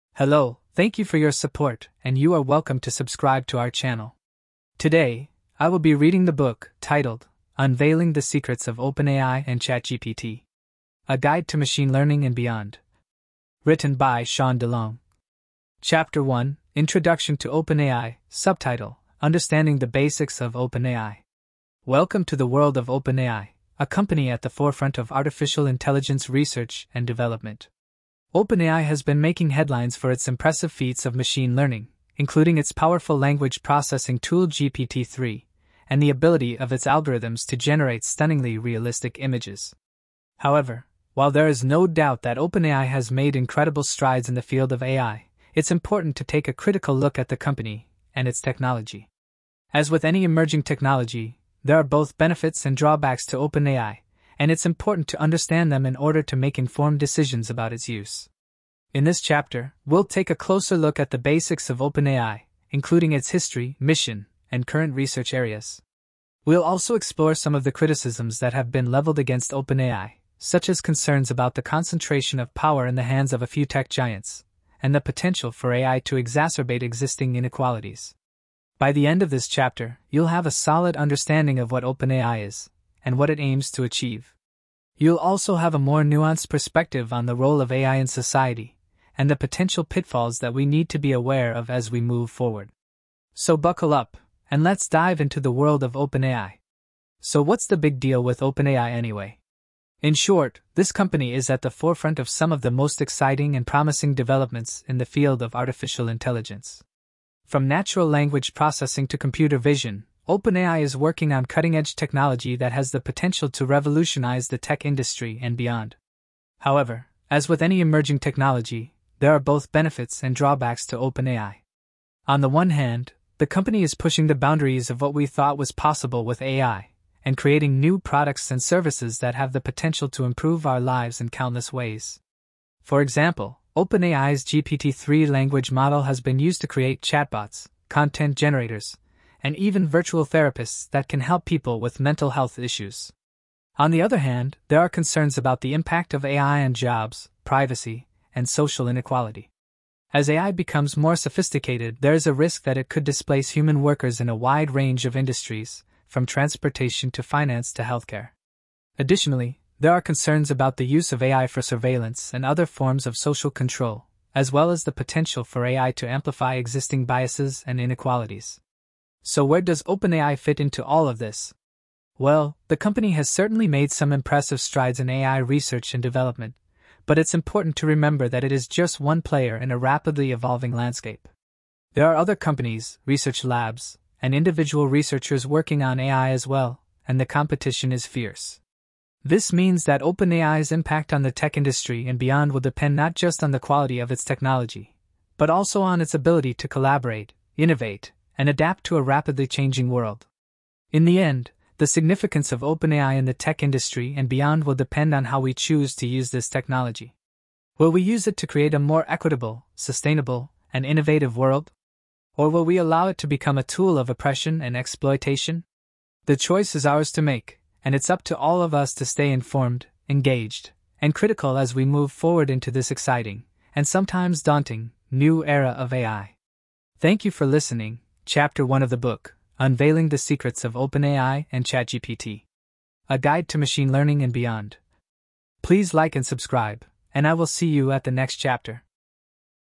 Unveiling the Secrets of OpenAI and ChatGPT: A Guide to Machine Learning and Beyond (01)
Chapter 1: Introduction to OpenAI
Understanding the Basics of OpenAI

Welcome to the world of OpenAI, a company at the forefront of artificial intelligence research and development. OpenAI has been making headlines for its impressive feats of machine learning, including its powerful language processing tool GPT-3, and the ability of its algorithms to generate stunningly realistic images.